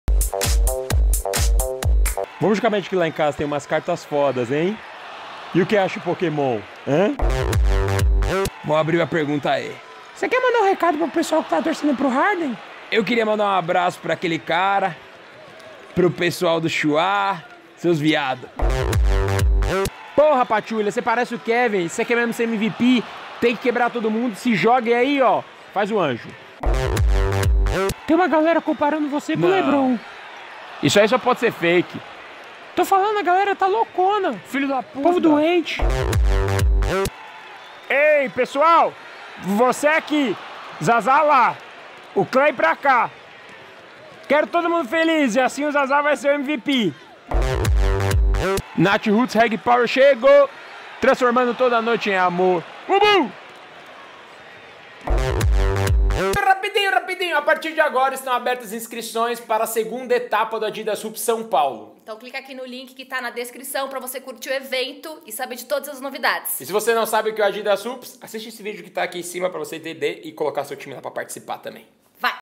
Chublagem #9 | Canal Chuá
Chublagem voltou, com o MVP da temporada e com comparações do LeBron James e Michael Jordan.

Produção: Mafia Produtora
Direção: Carolina Loureiro
Edição: Carolina Loureiro
Câmera: Carolina Loureiro
Roteiro: Gabriela Neves
Apresentador(es): Gabriela Neves / Bruno Marques

canalchua